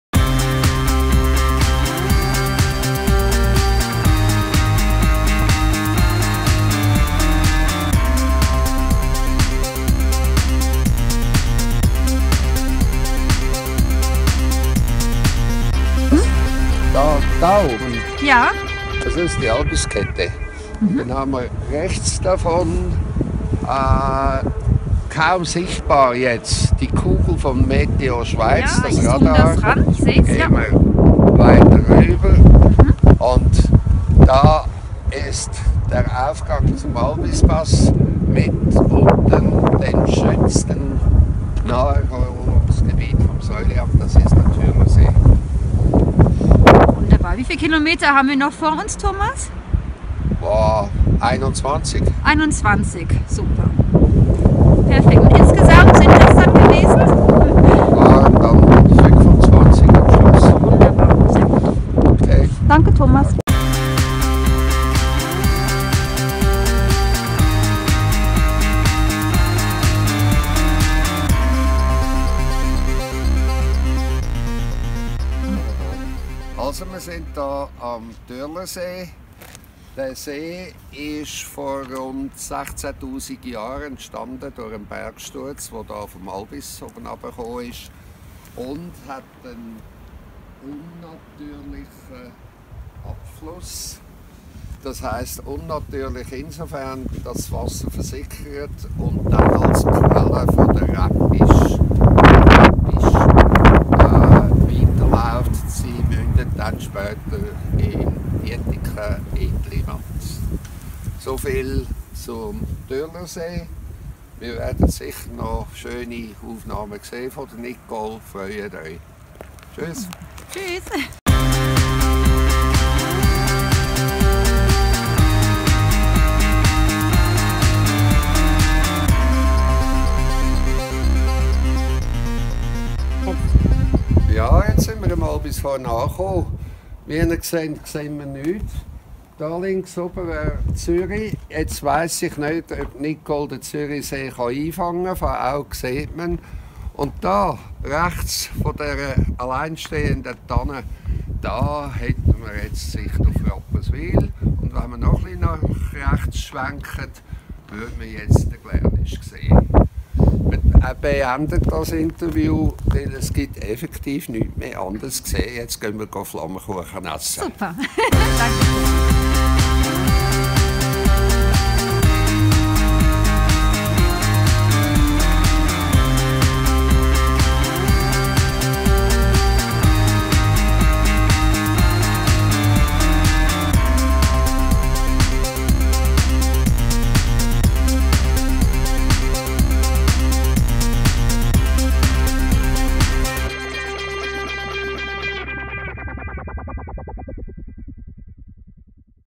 Frostwanderung - Thomas Auer - Wanderfreaks
24.02.18: Frost-Rundwanderung zu drei Highlights im Zürcher Säuliamt Affoltern am Albis – Türlersee – Albishorn – Oberes Jonental – Affoltern am Albis

nachgelesen werden.

25 km 650 hm.

Wanderer: Thomas Auer, Wilfried Mende und Nicole Hammer